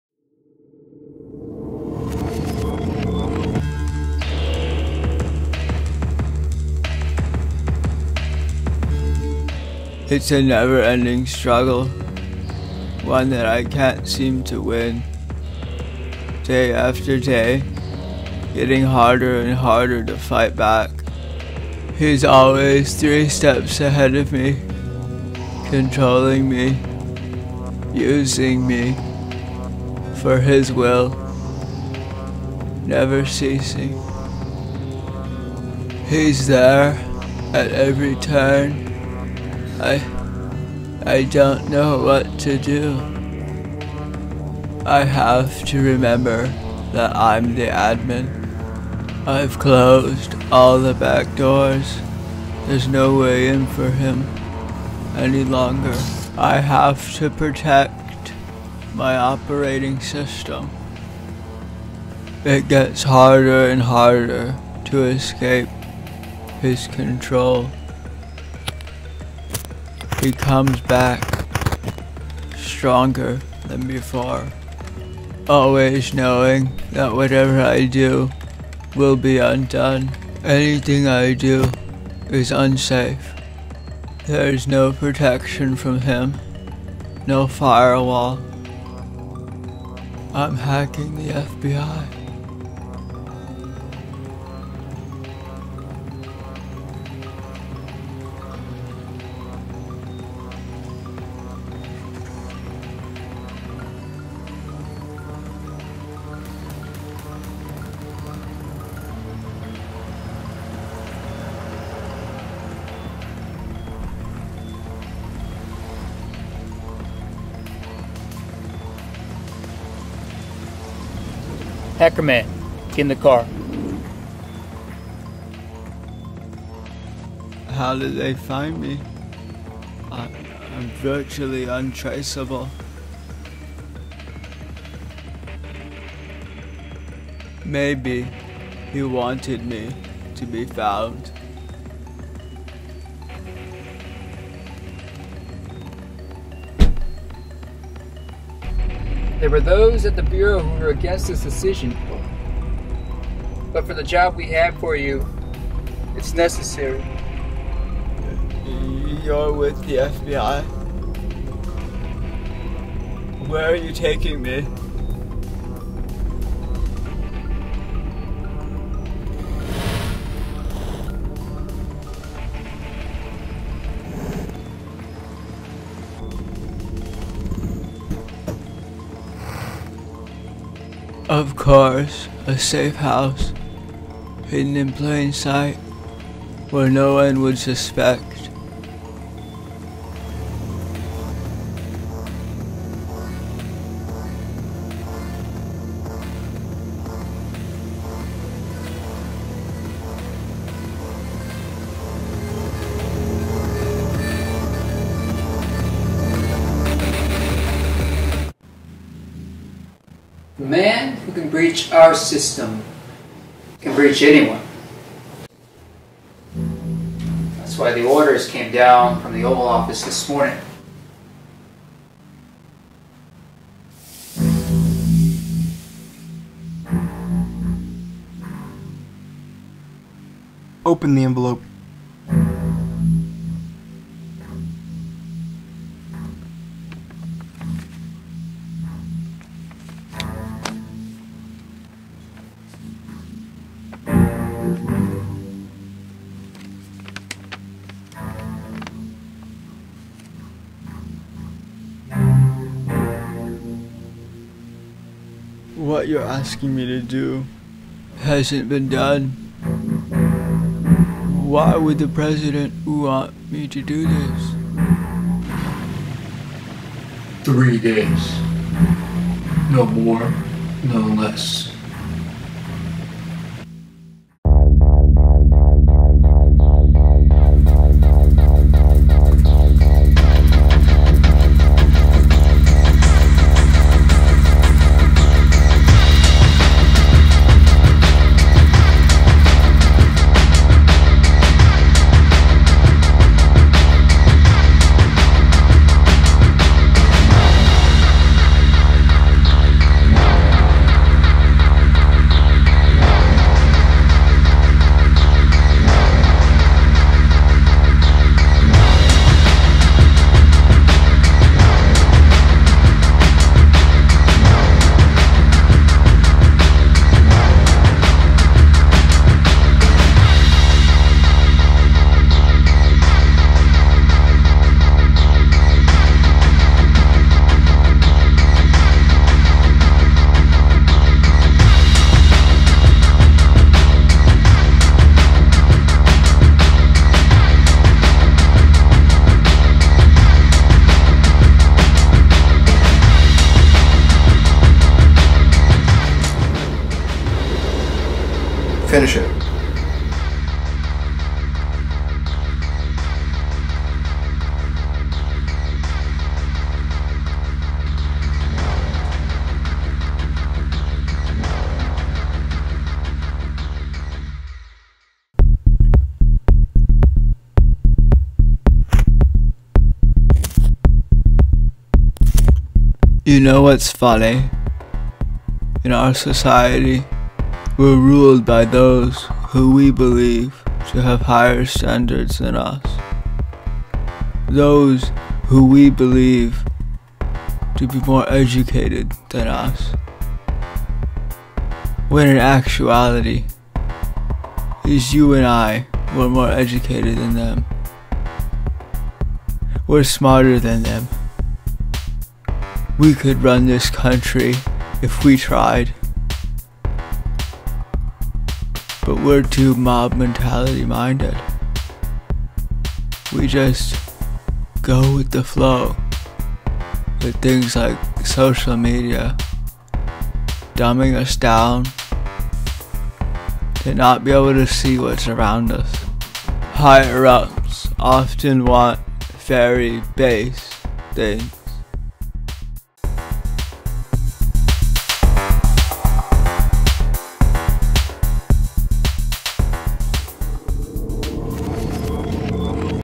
Hackerman: Vctry_Ryale.mp4
Hackerman is a homage/parody of the popular USA television series called 'Mr. Robot.' We hope you enjoy!